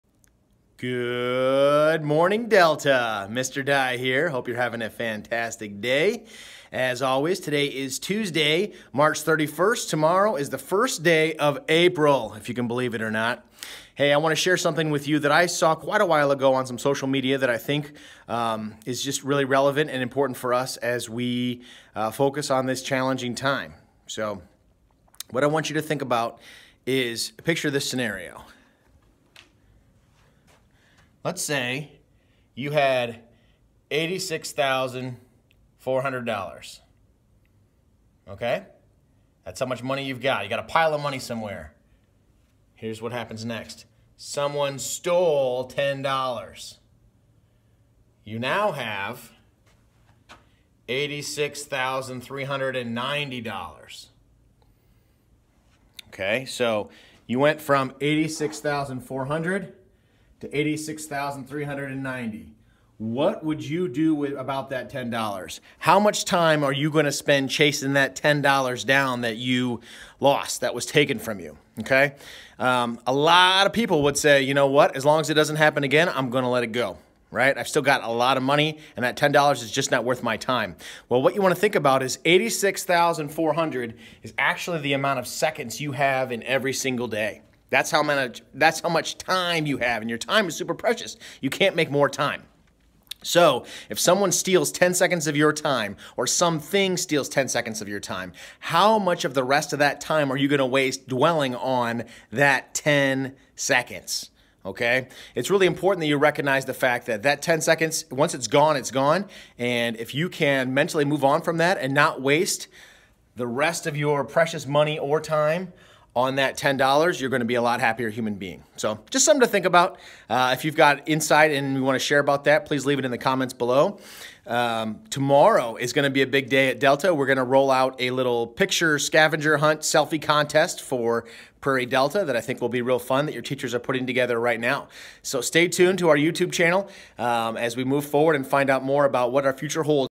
Delta Video Blog 3.31.2020 - Someone Stole $10
Sharing a concept today about your most precious commodity. - TIME